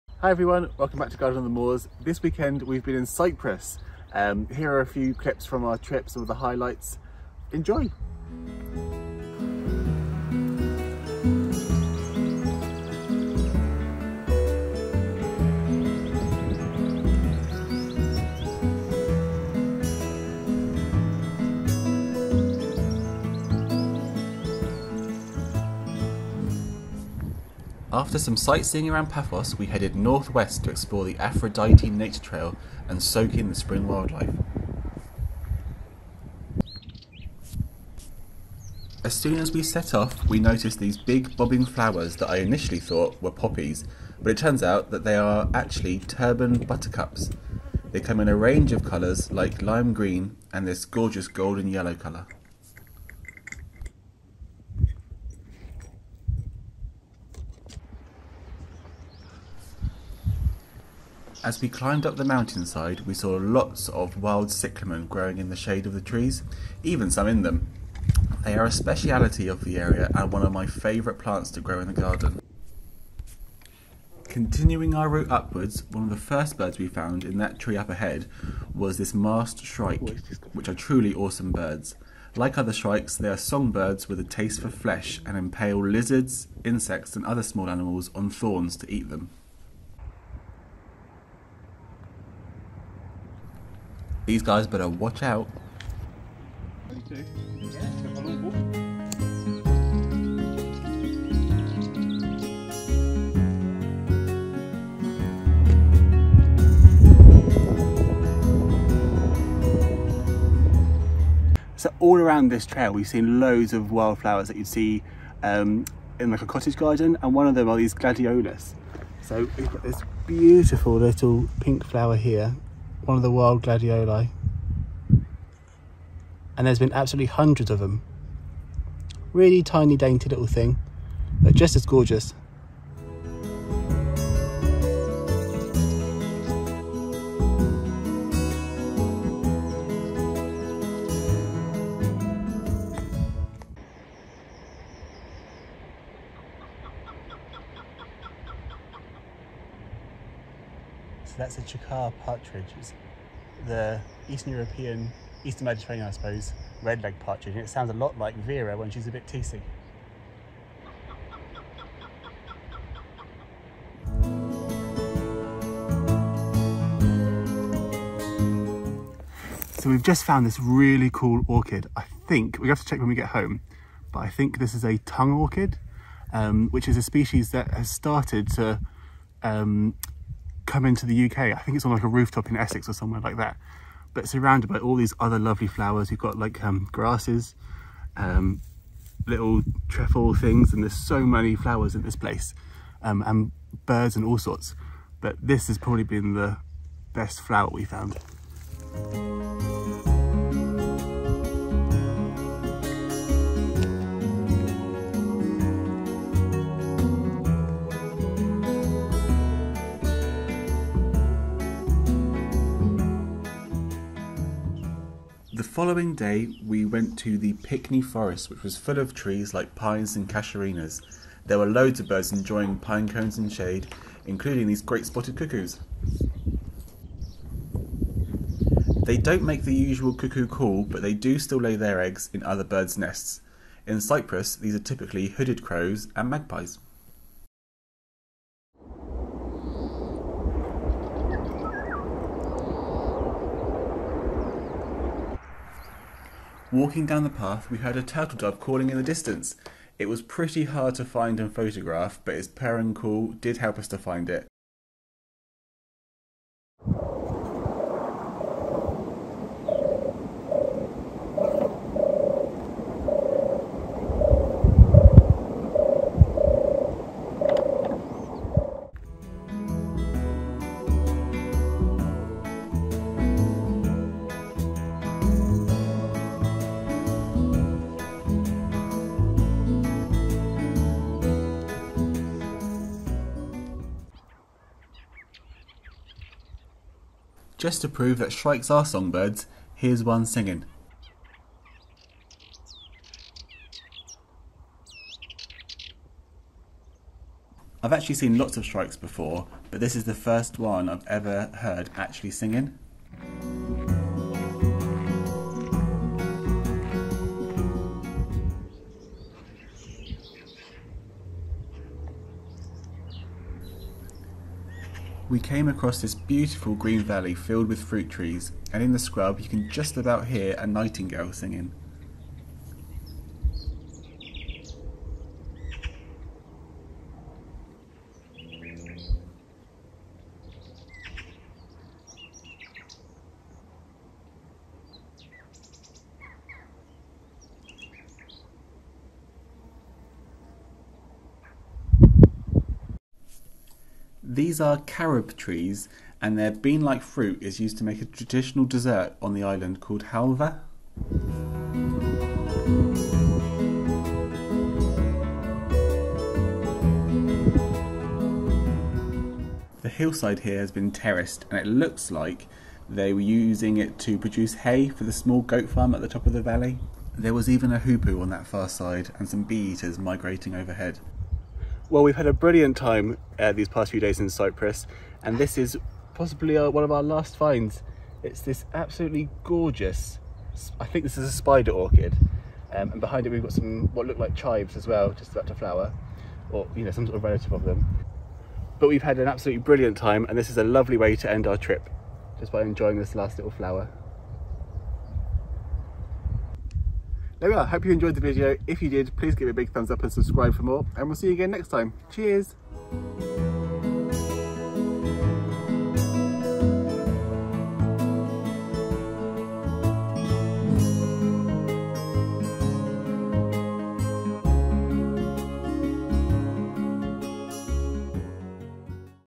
Wildflowers and Birdlife of Cyprus
Garden on the moors are on tour! In Cyprus for the Easter bank holiday weekend, we explored the Aphrodite Nature Trail and Pikni Forest to look for birds on spring migration and wildflowers like Cyclamen persicum, Gladiolus triphyllus and a whole host of unusual wild orchids. We find turtle doves, singing nightingales and a cuckoo like no other.

Host: Richard Hollis

🌼 CHAPTERS
00:00 Introduction
00:26 Aphrodite Nature Trail
00:37 Turban Buttercups
01:03 Wild Cyclamen
01:13 Masked Shrike
01:51 Wild Gladioli
02:24 Chukar Partridge
02:49 Tongue Orchid
03:35 Pikni Forest
03:43 Great Spotted Cuckoo
04:16 Turtle Dove
05:08 Nightingale
05:43 Carob Tree Orchard
06:12 Ophrys mammosa Bosom Orchid Spider Orchid